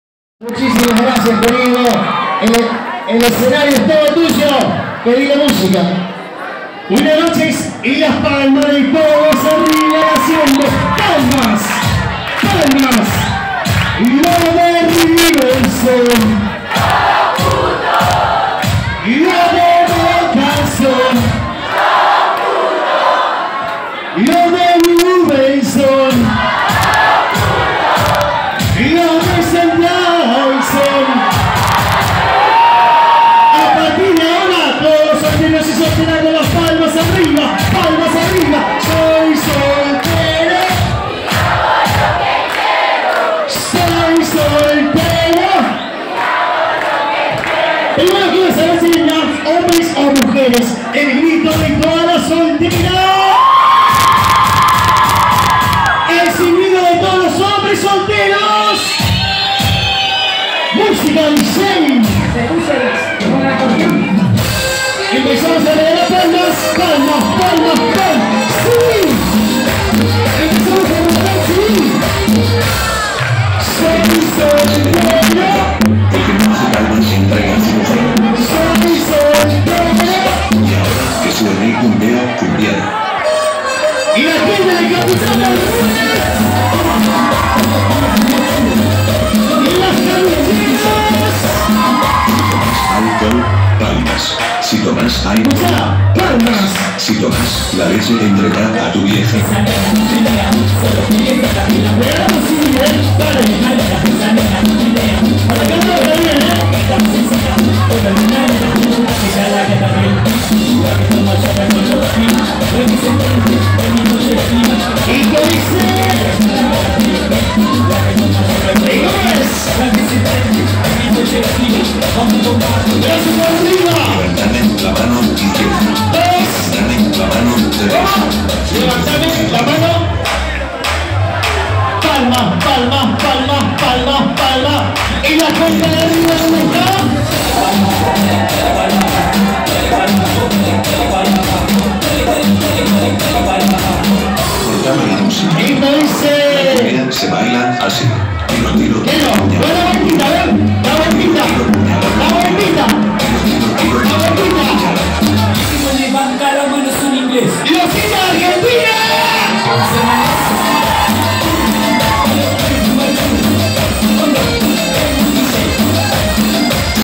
Dj Leo Ramos animando en Ucrania 26/07/29017
Mi Animacion en Ucrania ..gracias al locutor  Alejandro Made ... Victor Hugo Pintos por su buena honda